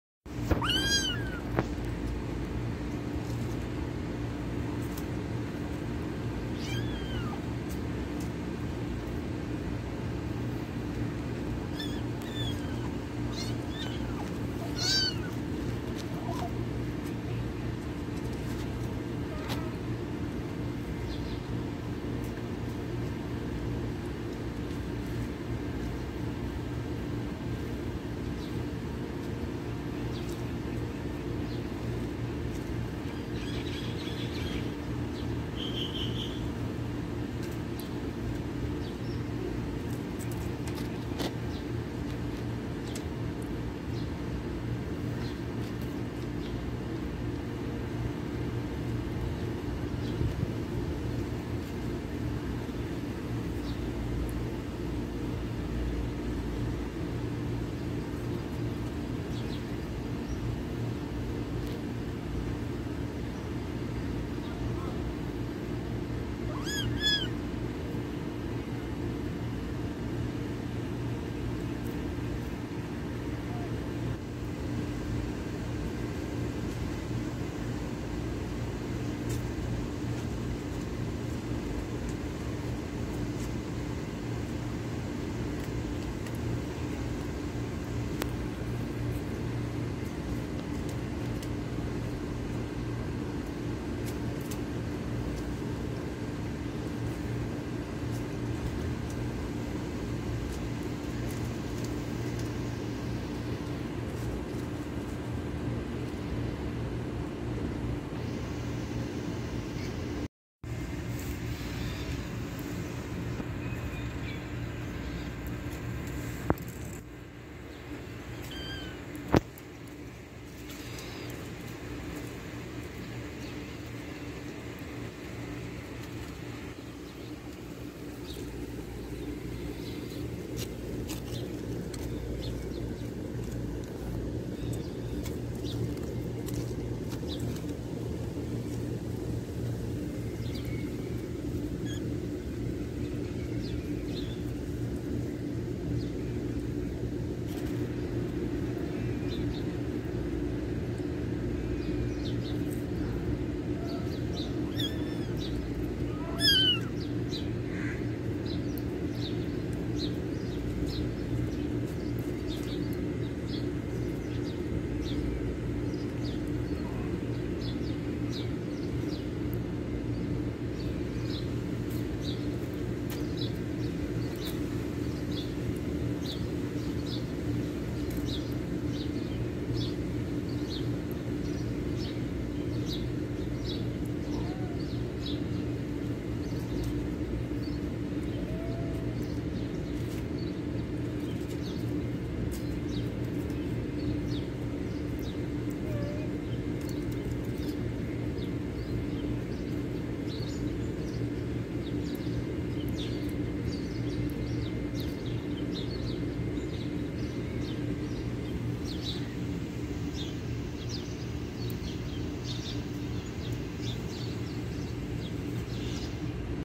A beautiful Preston cat and kitten playing in open cage#viralvideo lovely Kitten
Persian baby kitten playing! Beautiful Baby cate.